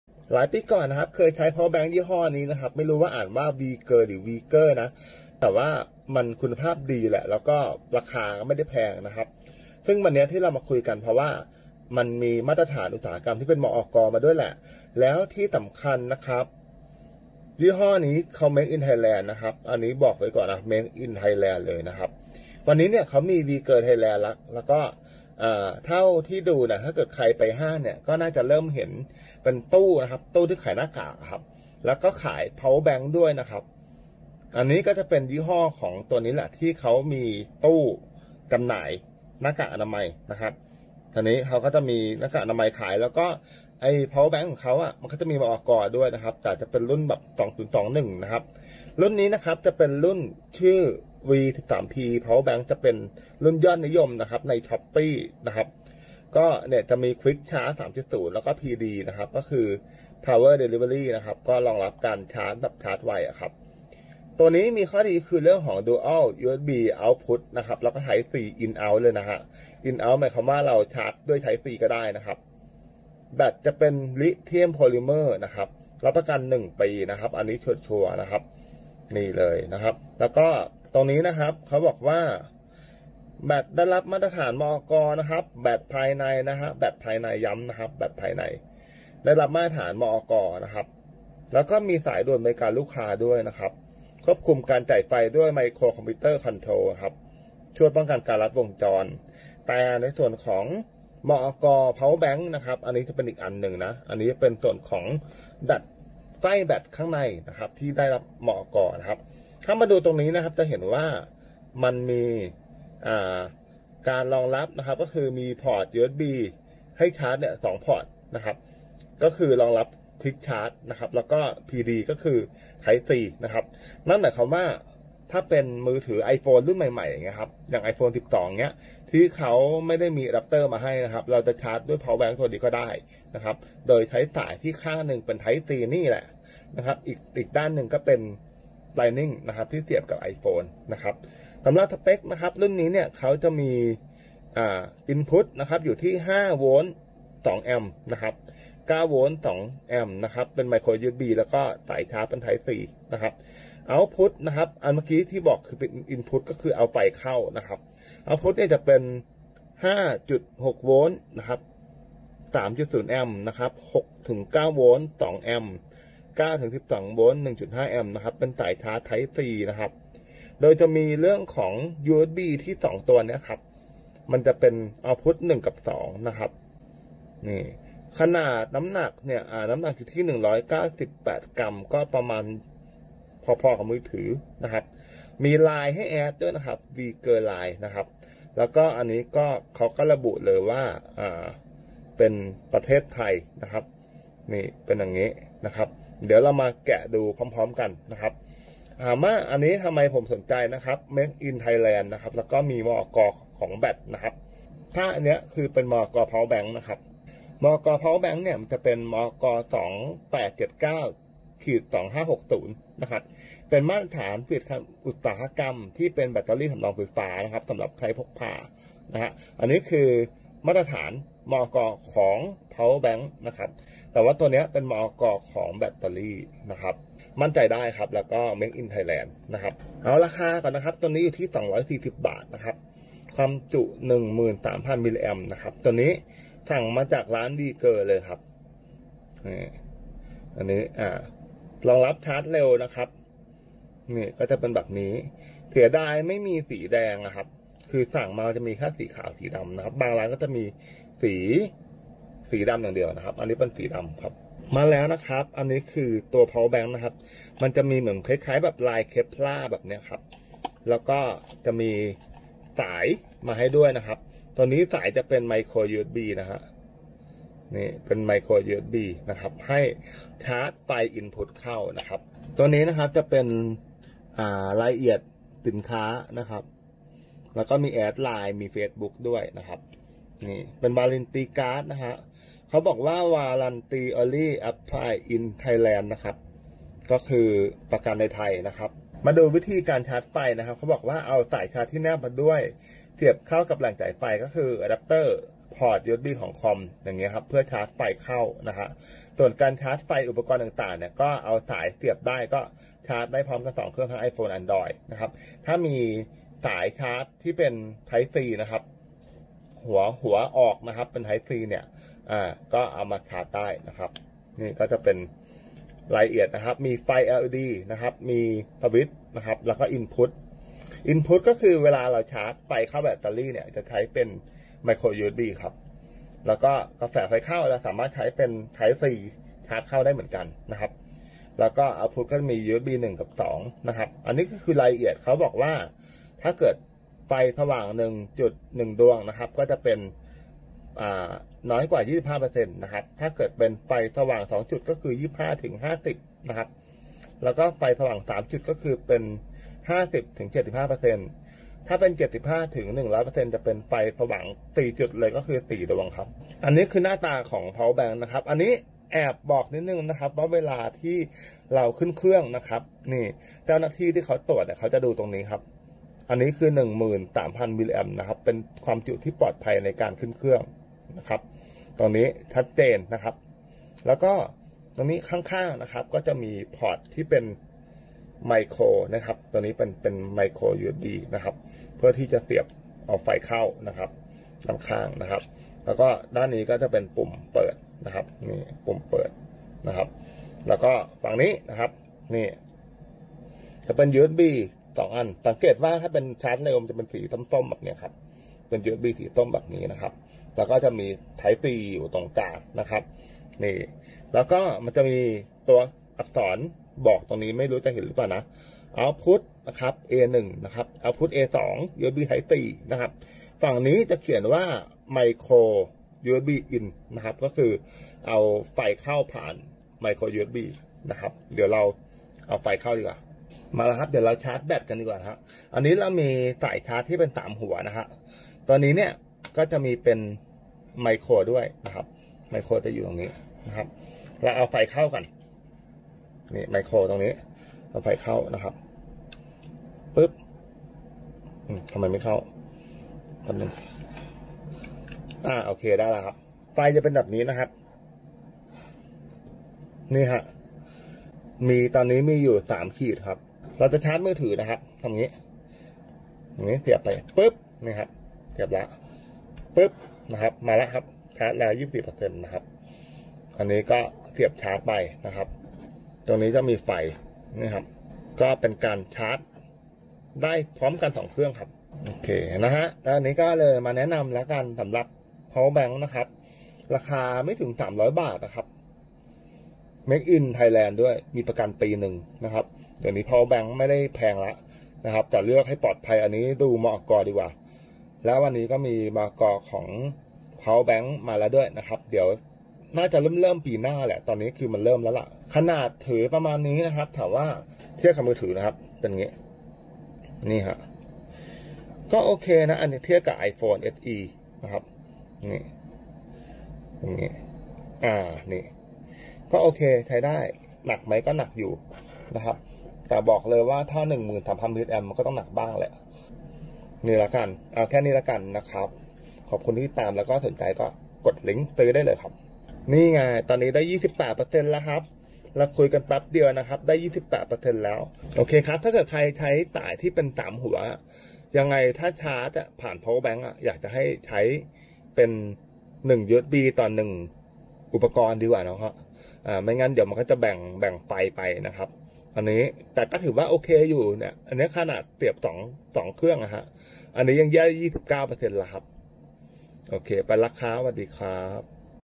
รีวิว แบตสำรอง veger V13P ทำในไทย จุไฟ 13000mAh ชาร์จได้ 2 เครื่องพร้อมกัน รองรับ PD ราคาหลักร้อย
ปัจจุบันรุ่นในวีดีโอ ไม่มีขายแล้ว หรือหายากมาก มีรุ่นใหม่มาแทน รองรับ 20W PD ชาร์จ iPhone 14 ได้ มีตัวเลขบอกระดับแบต (รุ่นเก่ามีไฟบอกอย่างเดียว) แต่รุ่นเก่าก็ใช้มาประมาณ 2 ปีแล้ว ยังใช้งานได้ดีอยู่

แบตสำรองหลักร้อย Made in Thailand มี มอก. ยี่ห้อ VEGER รุ่น P13PD PowerBank 10000mAh รองรับการชาร์จเร็ว 20W PD รับประกันสินค้า 1 ปี ชาร์จกับสาย iPhone 14 ได้ กดซื้อ

รองรับชาร์จเร็ว Quick Charge 3.0/2.0 + Apple PD + Fast Charge Power Bank